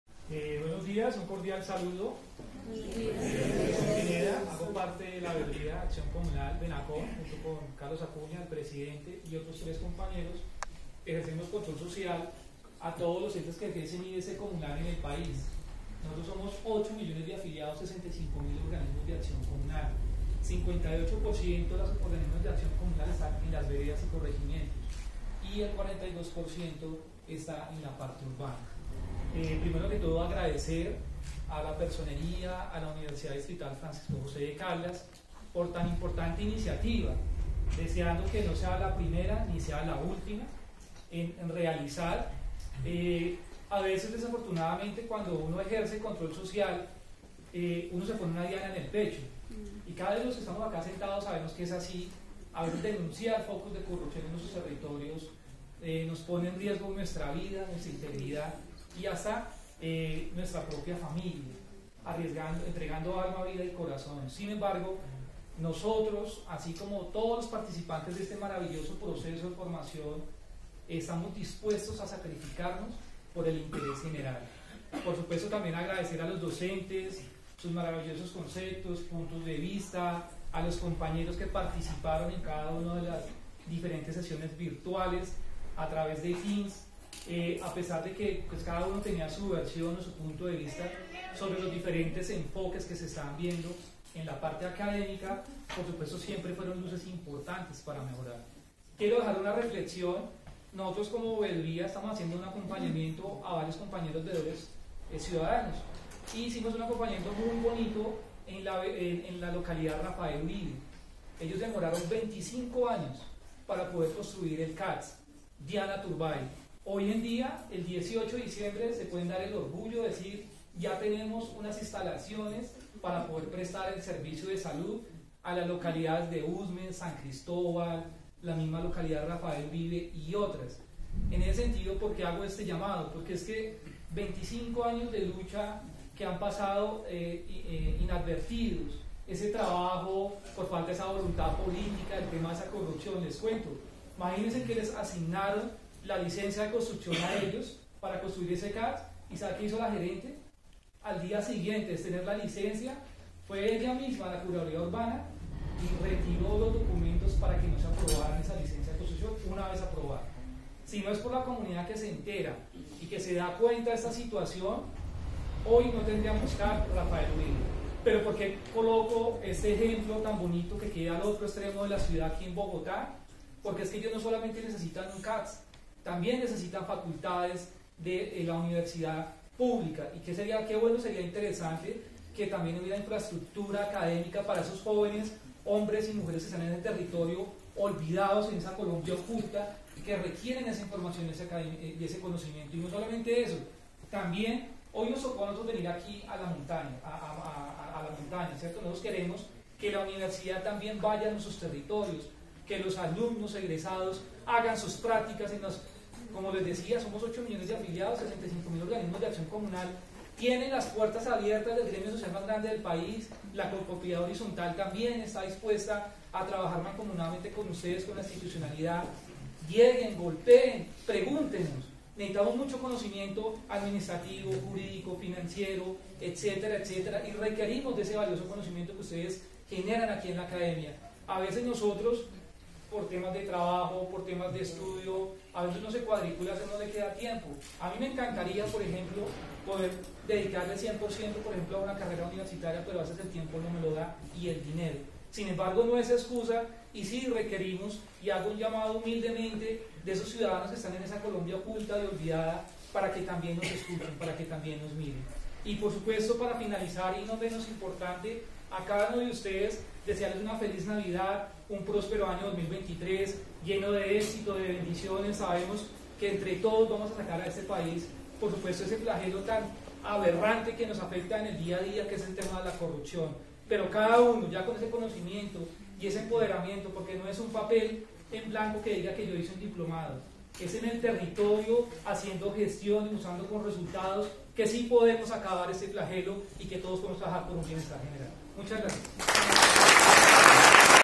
Intervención - Wilson Enrique Pineda Veedor Nacional - VENACOM «Diplomado Virtual»
Desde la «Veeduría Nacional de la Participación y Acción Comunal – VENACOM» se genera seguimiento y acompañamiento a las diferentes acciones desarrolladas en la comunidad y que afectan directa e indirectamente a tod@s nuestros afiliados de la Acción Comunal, en esta ocasión participamos activamente en el «Diplomado Virtual en participación ciudadana, prevención y control a la Función Pública» el cual fue realizado en alianza estratégica con la Personería Distrital de Bogotá D.C., y por el Instituto de Extensión para el Trabajo y el Desarrollo Humano IDEXU de la Universidad Francisco José de Caldas, según Contrato CI 138-2022, que permitió llevar a cabo este proceso de formación académico, adelantado de manera satisfactoria para los ciudadanos interesados en la ciudad Capital, el cual se realizó gracias a un arduo proceso de convocatoria con el fin de llegar a la mayor cantidad de ciudadanos y habitantes de la ciudad, interesados en conocer a profundidad temas relacionados con el Control Social que brindan herramientas para ejercer el mecanismo de la Veeduría Ciudadana.

Los participantes han sido protagonistas activos del proceso de enseñanza, aprendizaje, y durante el desarrollo del Diplomado se fomentaron diferentes tipos de espacios y participación, donde tod@s los beneficiarios pudieron acceder al contenido de manera fácil, asertiva y empática, con espacios asincrónicos propendiendo por la interacción grupal, así mismo espacios lúdicos de aprendizajes de los encuentros sincrónicos a través de la retroalimentación constante entre los docentes y los estudiantes.

Es sumamente importante capacitar y lograr este tipo de espacios para tod@s las personas con el fin de lograr estrategias que promuevan la participación ciudadana y así prevenir, combatir este gran flagelo que es la corrupción.

Espacio en el cual previamente se convocaron 6.000 ciudadanos, se inscribieron 535 ciudadanos, de los cuales el 80% logro terminar satisfactoriamente el proceso de formación.

El Contenido Temático del Diplomado Virtual en participación ciudadana, prevención y control a la Función Pública se desarrolló en 6 módulos, a los cuales pueden acceder a los espacios asincrónicos subidos al portal de la Veeduría Nacional VENACOM, a través de los respectivos links:

Módulo 1 «Elementos contextuales de la administración pública»
• La administración pública en la constitución de 1991
• El derecho Administrativo y la Administración Pública
• Fuentes del Derecho Administrativo
• Organización administrativa del Estado
• Entidades territoriales
• Taller análisis de casos.

Módulo 2 «Estatuto Anticorrupción»
• Marco conceptual de la corrupción
• Marco jurídico legal, supralegal e internacional
• La corrupción pública y privada
• Estatuto anticorrupción (Ley 1474 de 2011)
• Régimen disciplinario (Ley 1952 de 2019 Código General Disciplinario)
• Taller de análisis de casos.

Módulo 3 «Participación Ciudadana»
• E-Mecanismos de participación ciudadana
• Participación ciudadana inclusiva.
• Derechos humanos
• Enfoque de género y Representatividad
• Paridad de género y su marco jurídico

Módulo 4 «Planeación»
• Naturaleza y propósitos de la planeación
• La planeación como función administrativa
• Principios de la planeación
• Tipos de planes
• Importancia estratégica de planeación

Módulo 5 *«Presupuesto público»*
• El presupuesto público
• Marco general del presupuesto
• Programación presupuestal
• Ejecución presupuestal
• Cierre presupuestal y fiscal

Módulo 6 «Sistemas de contratación pública en Colombia»
• Etapas de la Contratación Estatal, precontractual, contractual y pos- contractual
• Proceso de Selección de contratistas
• Sistema de Compras y Contratación Pública a partir del Decreto 1510 de julio 17 de 2013. CIIU Clasificador de las Naciones Unidas.
• Colombia Compra Eficiente. Plan Anual de Adquisiciones.
• El Sistema Electrónico para la Contratación Pública – SECOP I y II
• Funciones de la Agencia Nacional de Contratación Pública. Decreto 4170 de 2011 y Ley 1082 de 2015.

Desde esta tribuna de Control Social queremos agradecer el enorme esfuerzo realizado por Personería Distrital de Bogotá D.C., y por el Instituto de Extensión para el Trabajo y el Desarrollo Humano IDEXU de la Universidad Francisco José de Caldas, con todos los profesionales asignados en la realización de este importante proceso de formación en Control Social y lucha frontal contra la corrupción, a los docentes, alumnos, beneficiarios del mismo, el cual nos dan luces importantes desde el conocimiento, la experiencia, el trabajo en los territorios.